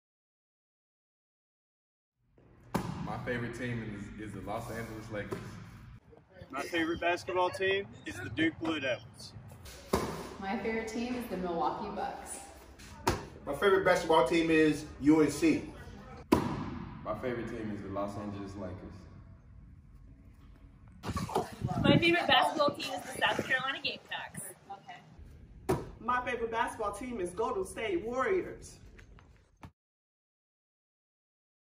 March Madness
Staff shares their favorite basketball teams with you!